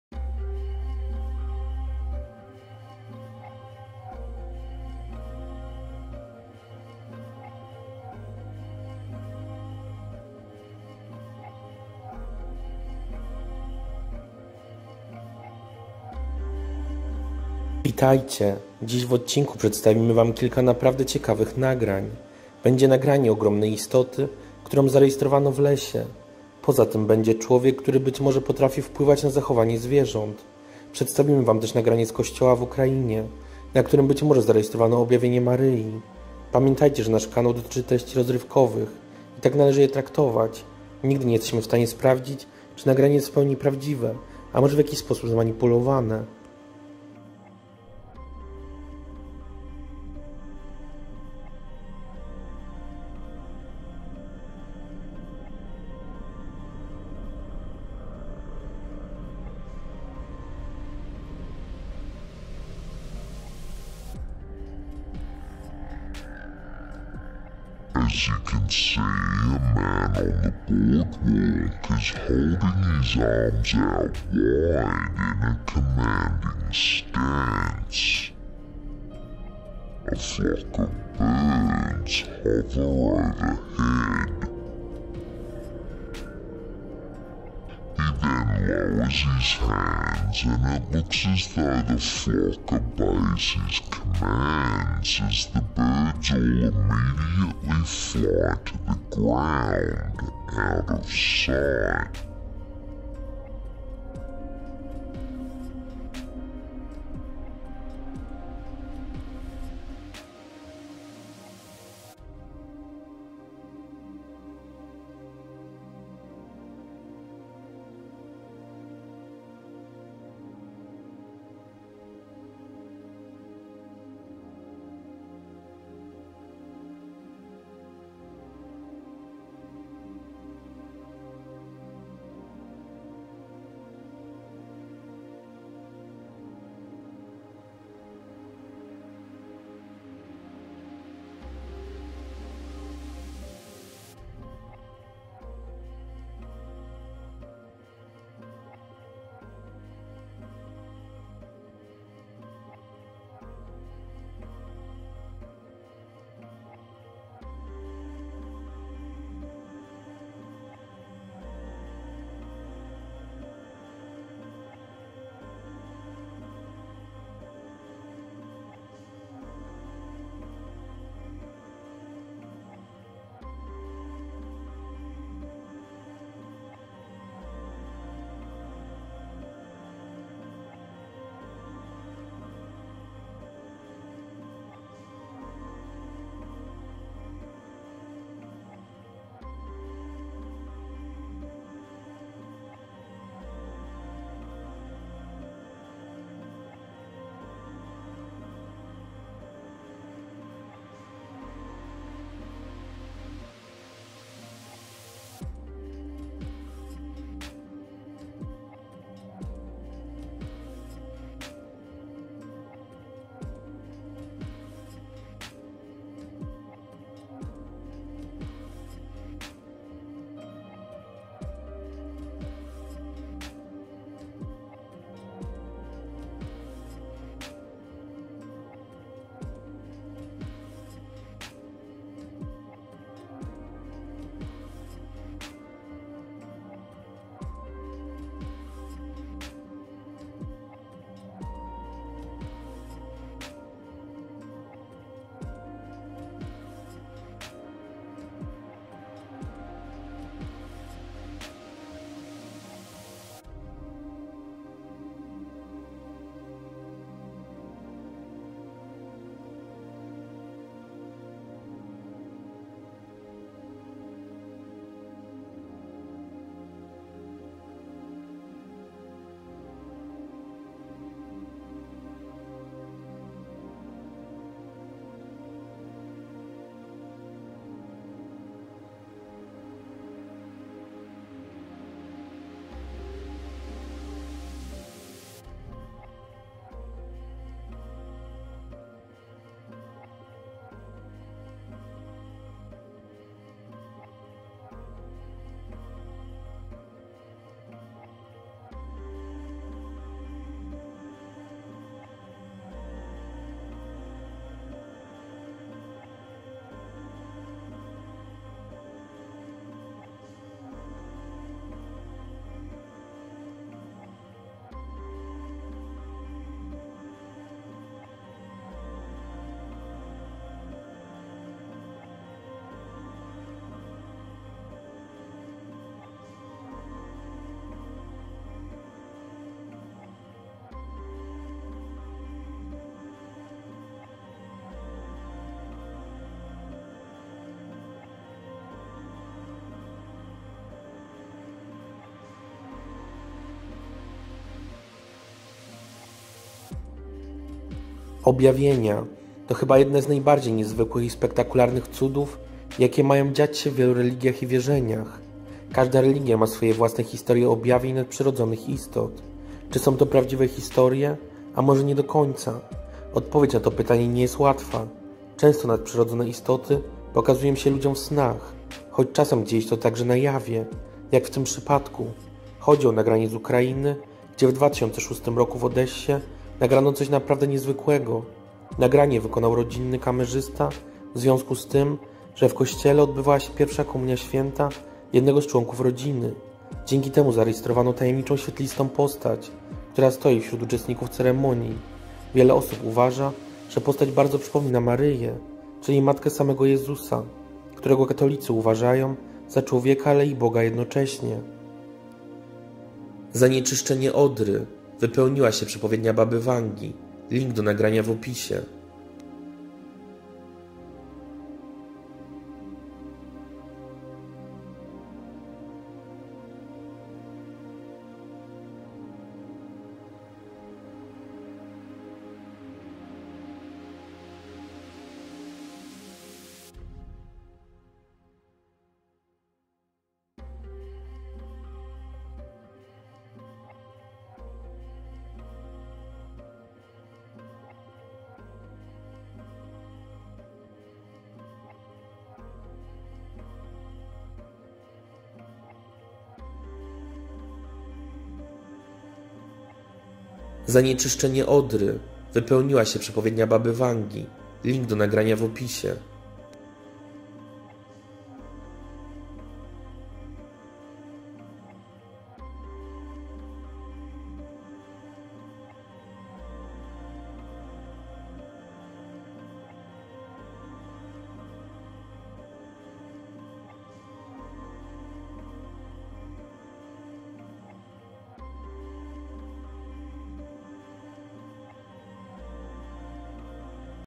Matka Boska objawia się w Kościele w UKRAlNlE? Nagranie zadziwia
Dziś w odcinku będą nagrania paranormalnych istot. Będą więc nagrania duchów  czy też mężczyzny, który panuje nad zwierzętami siłą umysłu.  Poza tym będzie nagranie Świetlistej istoty nagranej w kościele w UKRAlNIE. Czy objawiła się tam Maryja - Matka Jezusa - Czy to cud a Matka Boga objawia się na Ziemi?